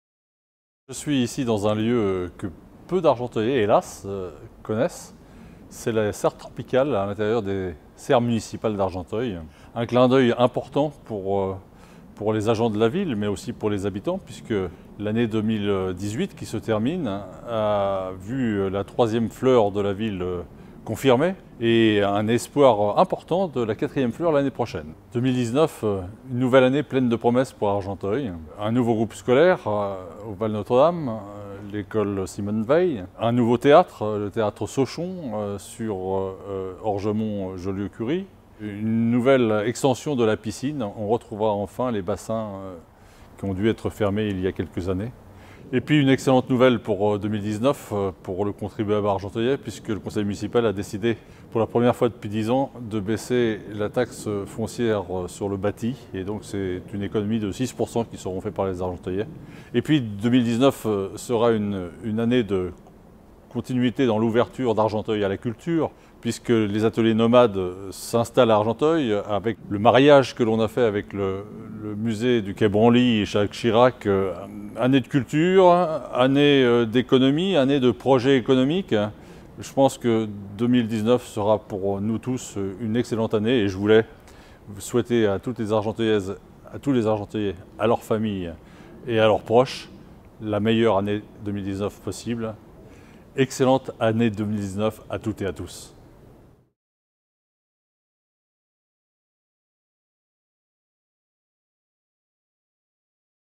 [ 2019 ] Les voeux du maire d'Argenteuil pour la nouvelle année
Georges Mothron, maire d'Argenteuil, souhaite une très bonne année aux argenteuillaises et aux argenteuillais.